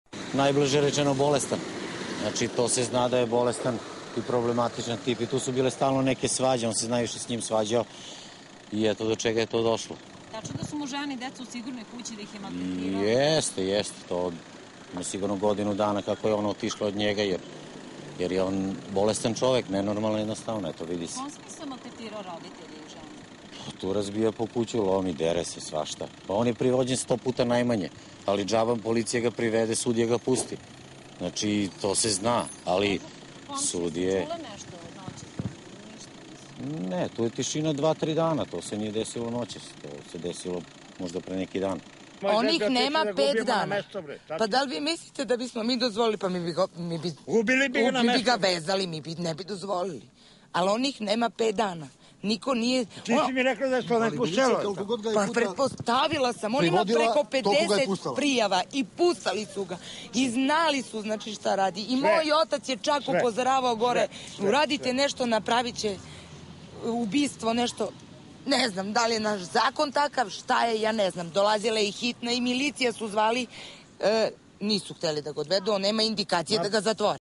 Niška Banja: Sin ubio roditelje
Na području Niške Banje dogodilo se dvostruko ubistvo. Kako je saopštila
policija, sin je ubio oca i majku i bacio ih u septičku jamu.